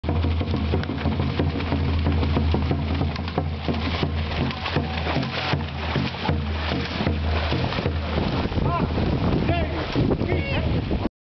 Dutch1 unleashed!
The Dutch dragons team 1 training in Liverpool